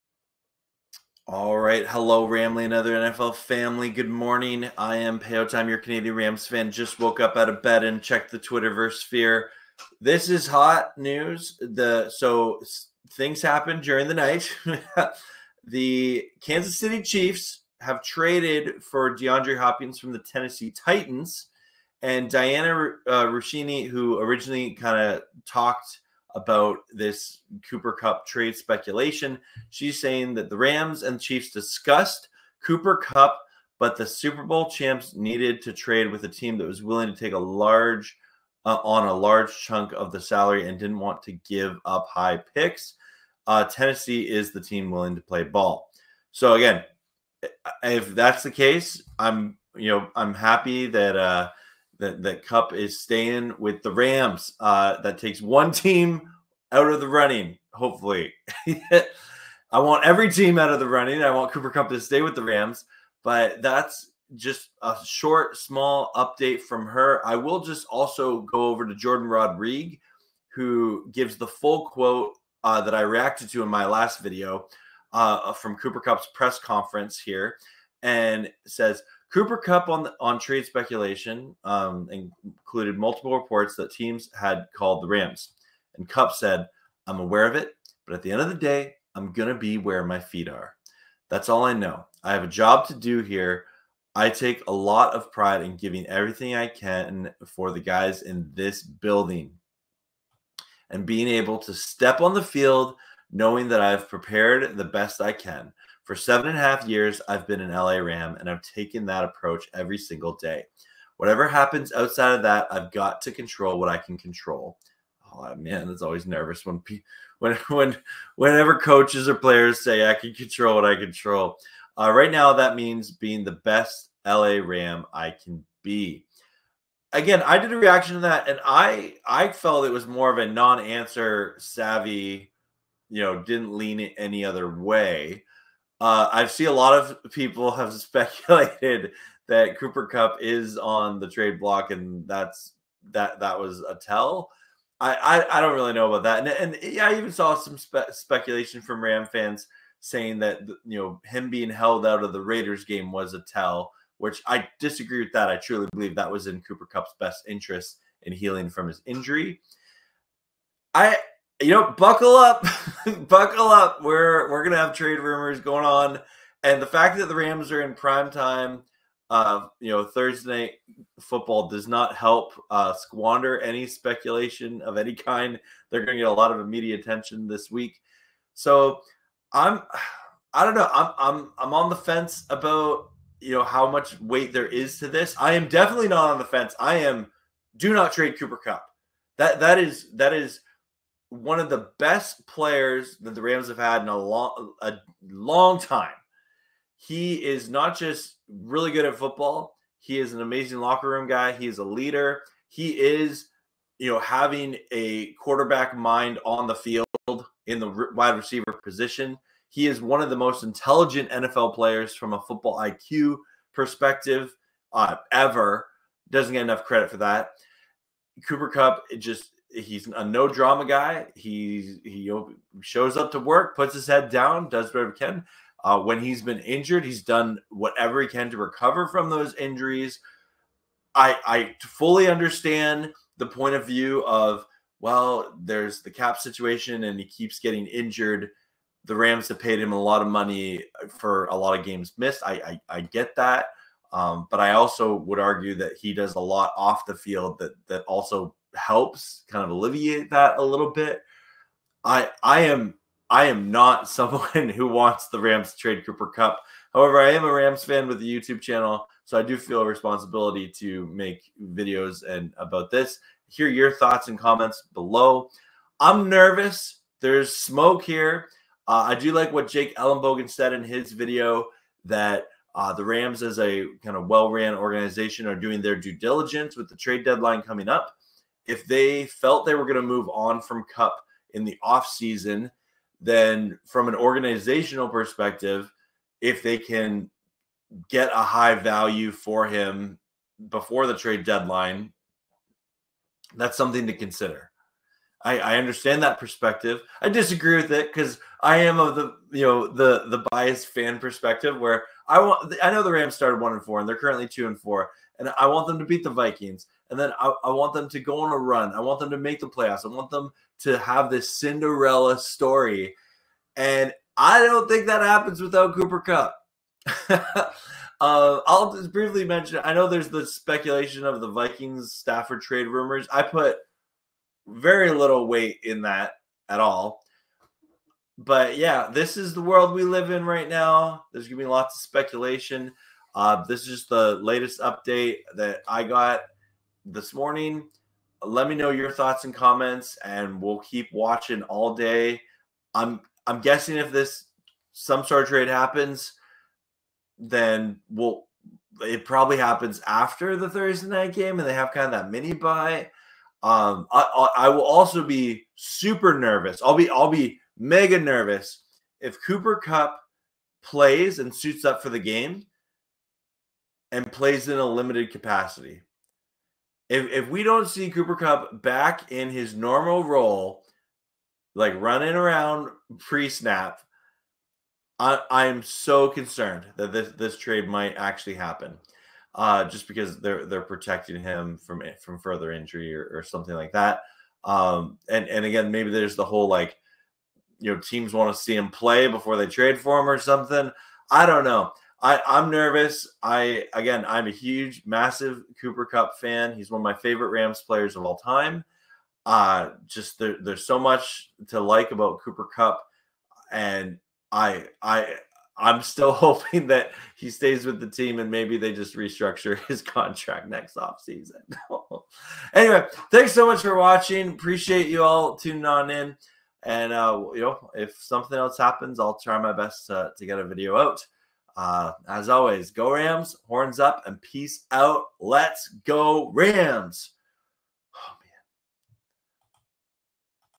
Hopkins to Chiefs! What does this mean for Cooper Kupp and the Rams? Latest LA RAMS NEWS
In this video, I react to the latest Cooper Kupp news! Cooper Kupp's latest press conference as trade speculation surrounds the LA Rams. Could one of the Rams' star players be on the move? I break down key moments from the presser, share my thoughts as a Rams fan, and discuss how these trade rumors might impact the team moving forward. Are the Rams preparing for a big roster shakeup?

Tune in for all the latest updates and analysis on the Rams' trade situation!

"Rams trade rumors," "Matthew Stafford press conference," "Cooper Kupp trade speculation," "Rams news," "NFL trade rumors."